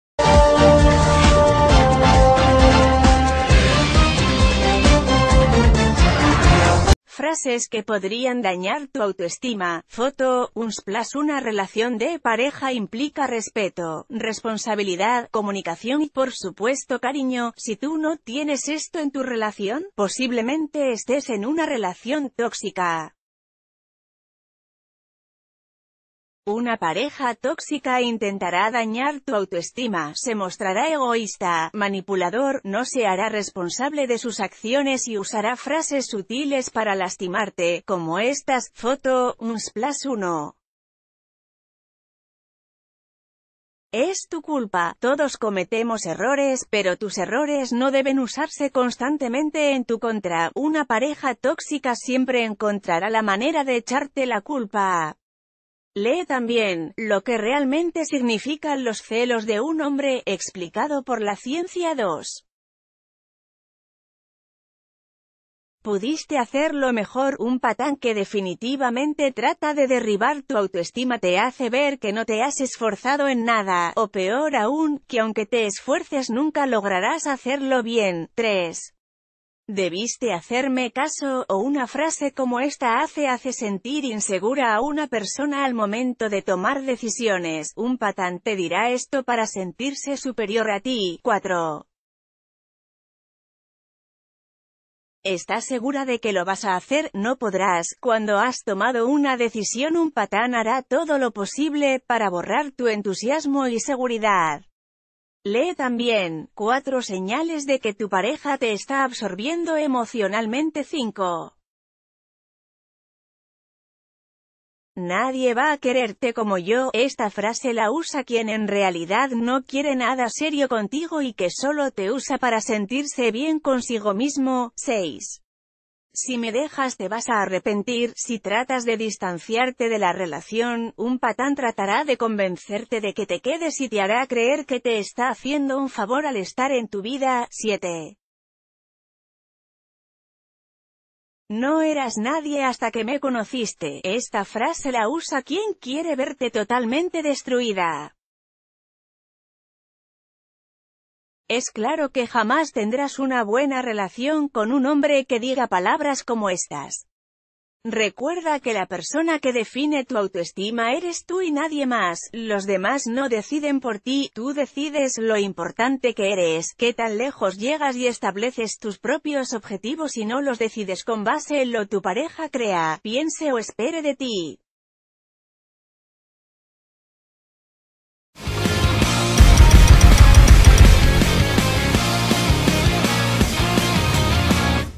Frases que dice un patán cuando no quiere nada serio contigo y que dañan tu autoestima
Frases que podrían dañar tu autoestima | Foto: Unsplash Una relación de pareja implica respeto, responsabilidad, comunicación y, por supuesto cariño, si tú no tienes esto en tu relación, posiblemente estés en una relación tóxica. Una pareja tóxica intentará dañar tu autoestima. Se mostrará egoísta, ...